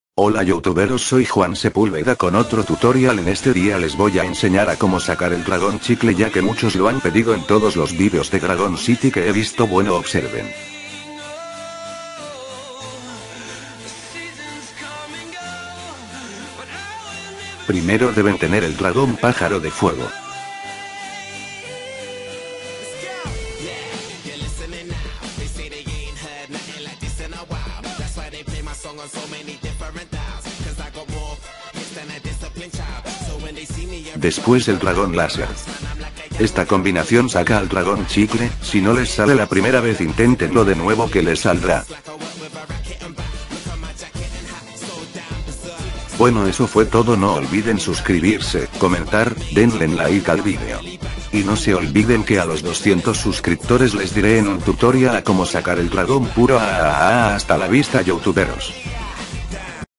Como Sacar Al Dragon Chicle [ Gummy] / Loquendo / HD 2012
Estas son Algunas Combinaciones q les Podra Salir:
Laser + Dandelion
Fluorescent + Firebird
Neon + Nenufar
Lantern Fish + Rattlesnake
Jade + Star
Plant + Star
Firebird + Star